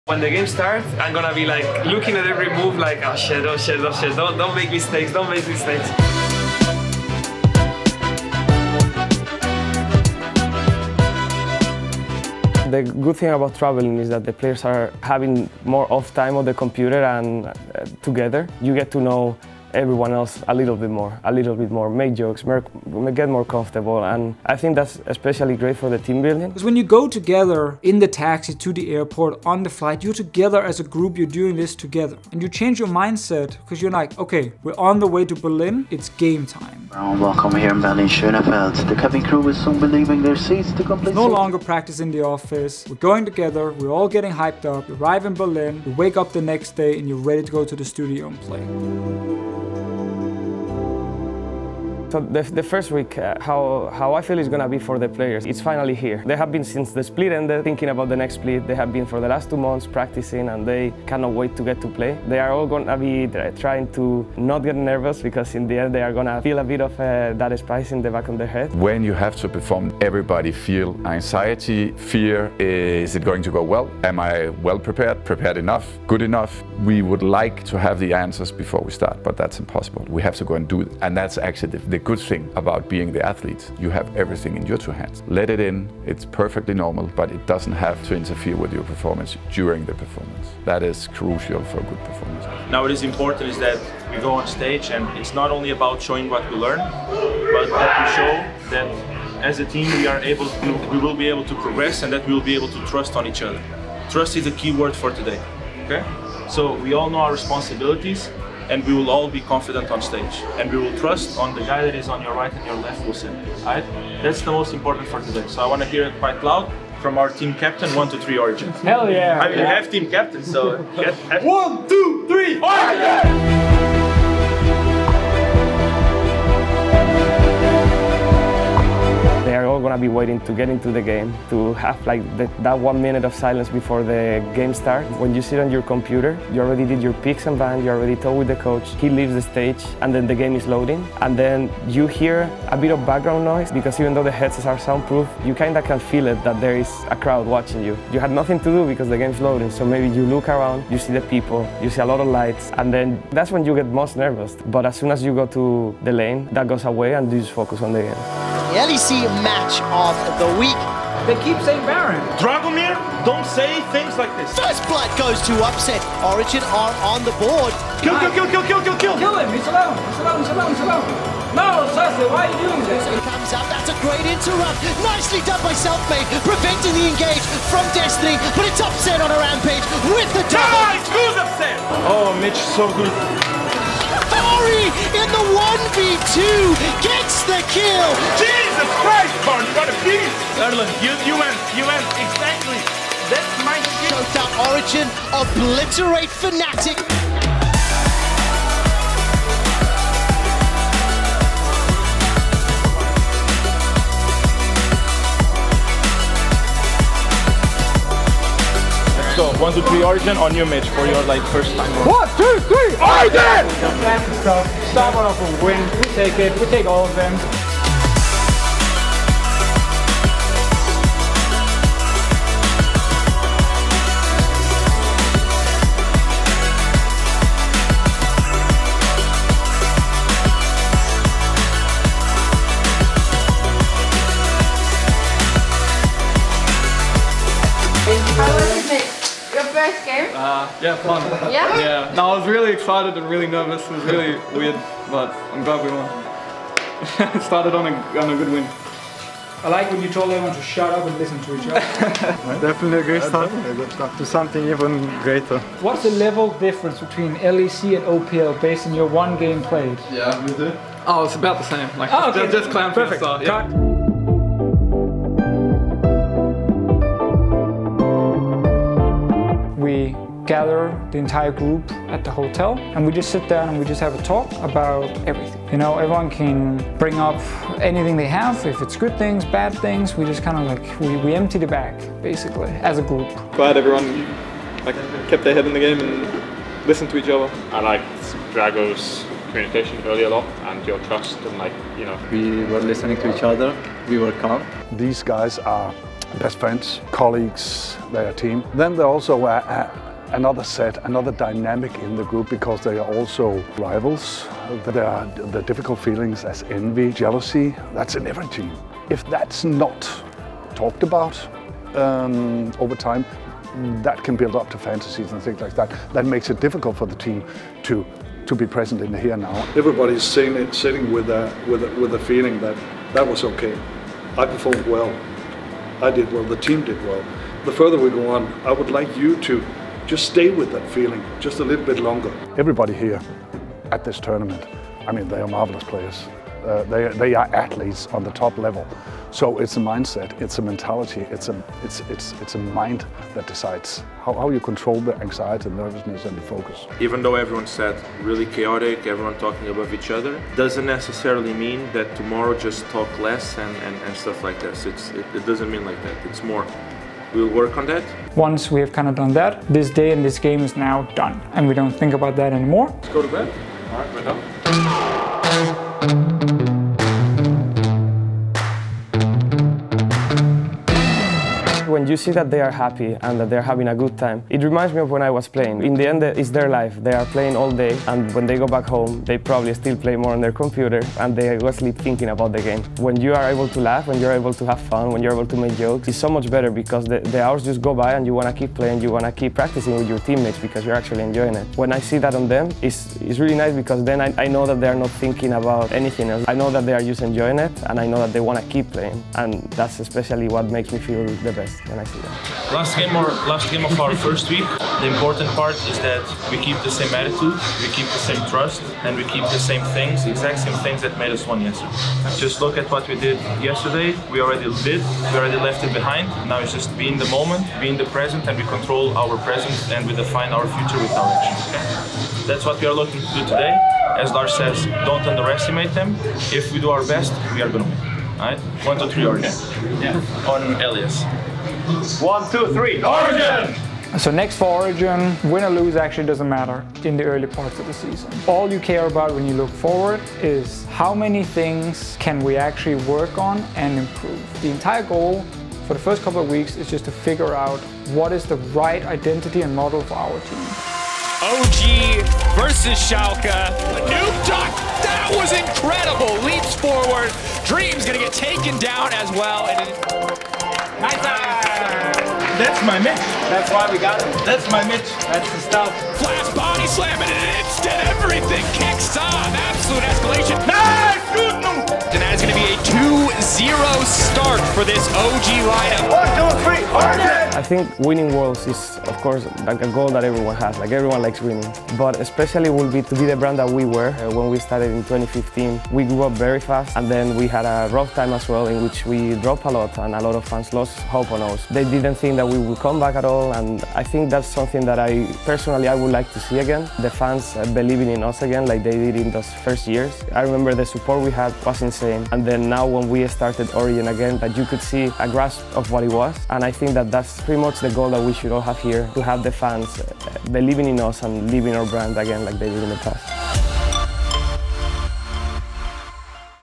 Backstage With xPeke and Origen at the LEC | Origen Documentary | Episode 3: The Great Start
You don't want to miss following Origen on them team's trip to Berlin for the LEC. Join them backstage for exclusive behind-the-scenes footage, and see how xPeke reacts to the games!
Twitter.com/Origengg
Instagram.com/Origengg
Facebook.com/Origengg
Twitch.tv/Origengg_